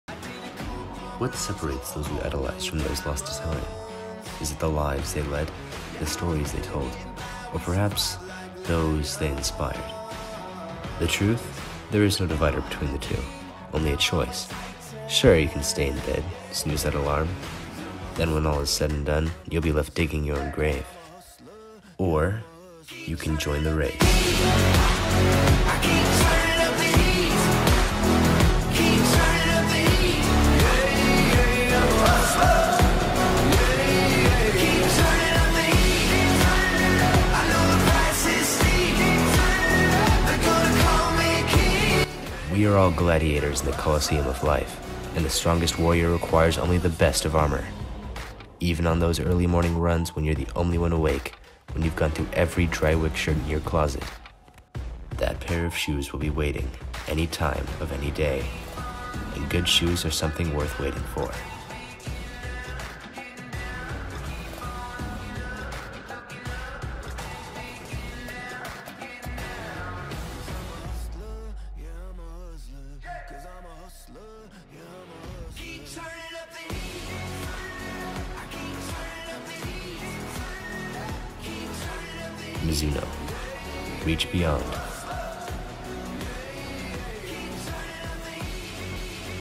Mizuno - Shoe Advert
The moody and stylish trainer advert was create, produced and filmed by Carter as part of his Advert assignment in Film class